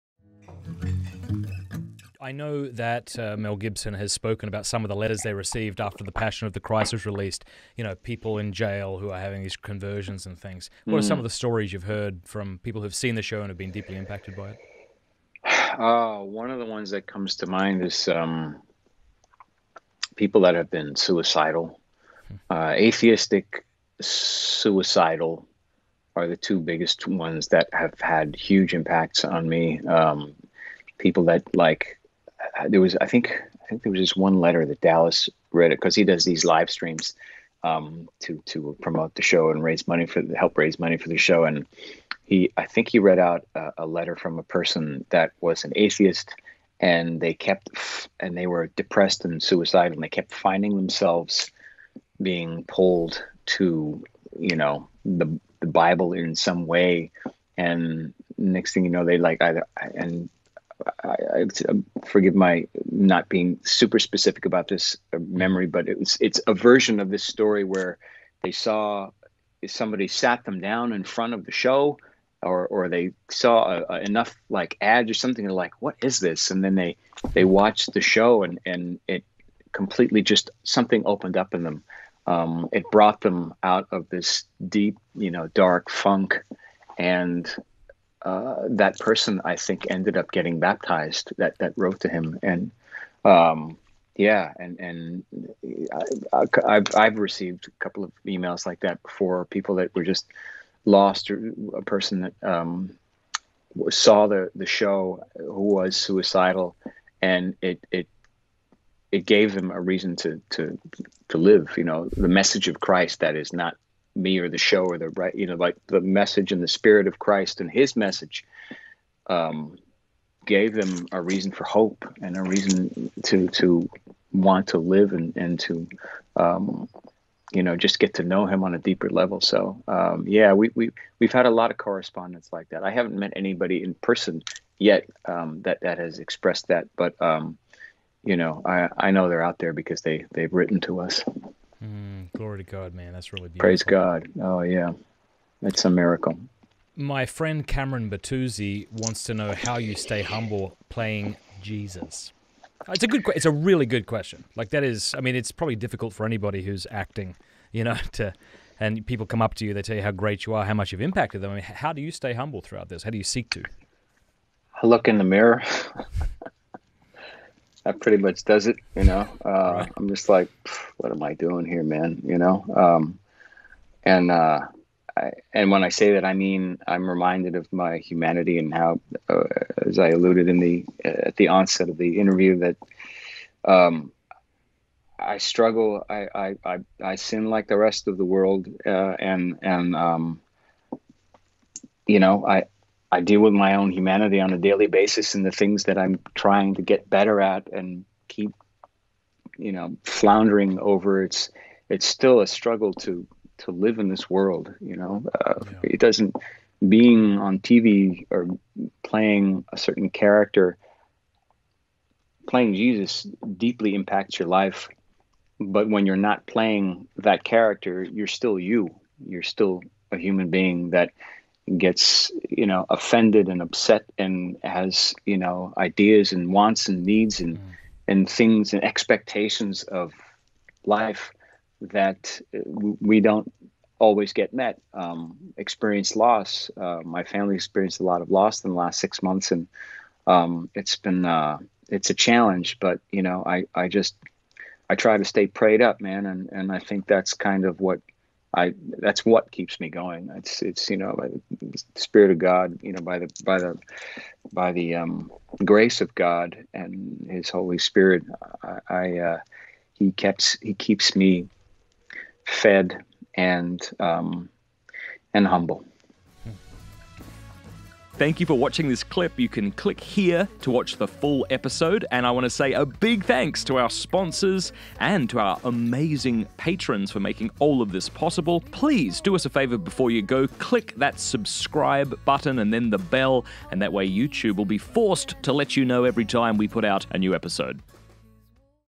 How to stay humble (while playing Jesus on TV) w/ Jonathan Roumie
In this clip, Jonathan talks about how he stays humble while playing the "King of Kings" on TV.

🔴 GIVING

🔴 LINKS

🔴 SOCIAL